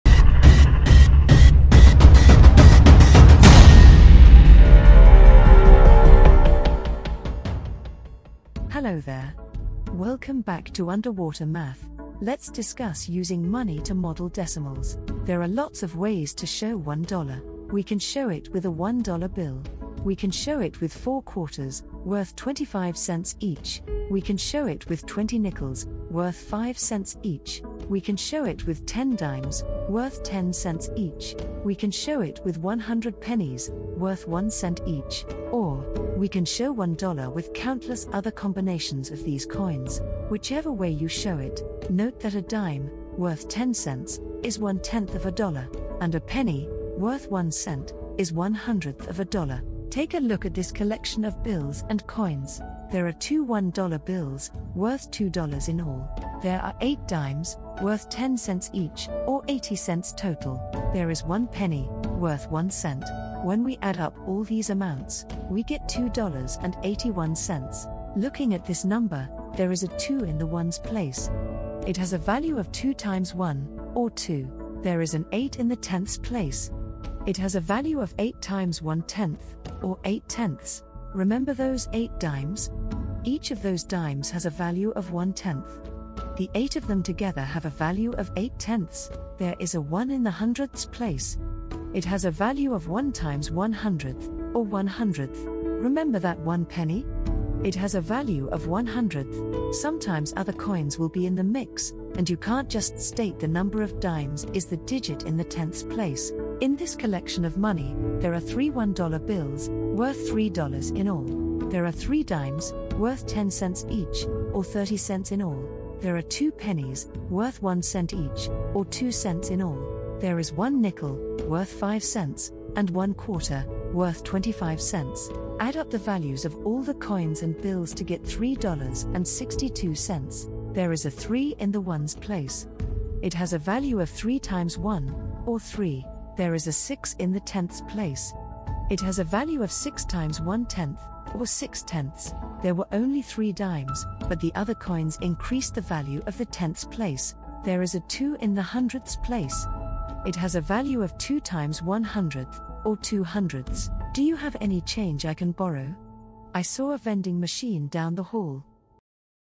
Decimal Models with Money - 4th Grade
Underwater Math provides engaging learning solutions for students. This video describes decimals using money as a visual model. Perfect for 4th grade students. TEKS 4.2.E